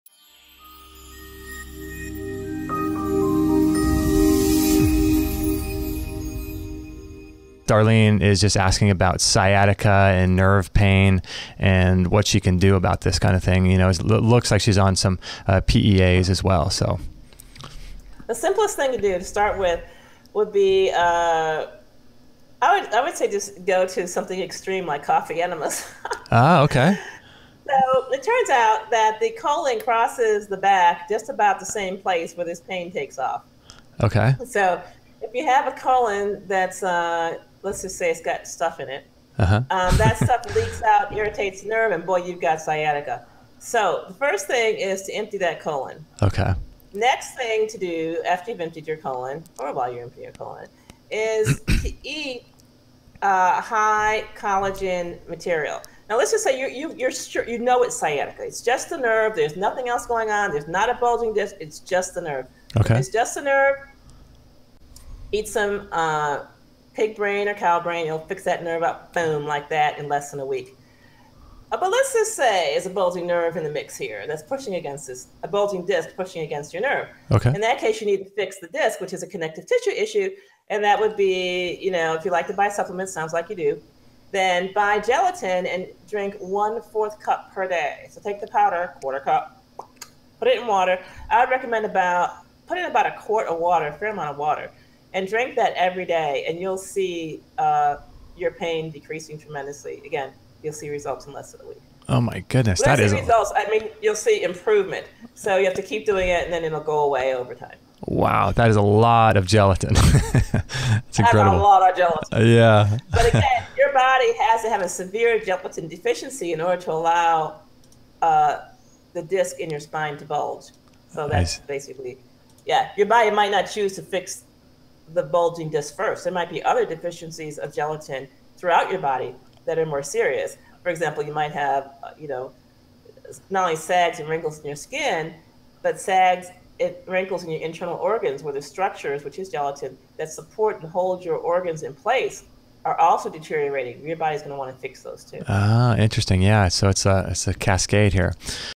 A Powerful Sciatica Treatment From Dr. Jennifer Daniels
Dr. Jennifer Daniels talks about a powerful sciatica treatment that you might not have heard before. Sciatica is where a person has a herniated disc that's bulging and impinging on a nearby nerve which usually radiated down the leg and causes extreme pain.

If you have a sciatica diagnosis and are wondering how long sciatica will last, you might be able to avoid sciatica pain medication or even surgery by not just doing exercises for sciatica but also these two powerful treatments for sciatica from Dr. Jennifer Daniels.

Click the links below to learn more!

Use code : EHR14 to start your free 14 day trial

Dr. Jennifer Daniels

Never Miss A Show!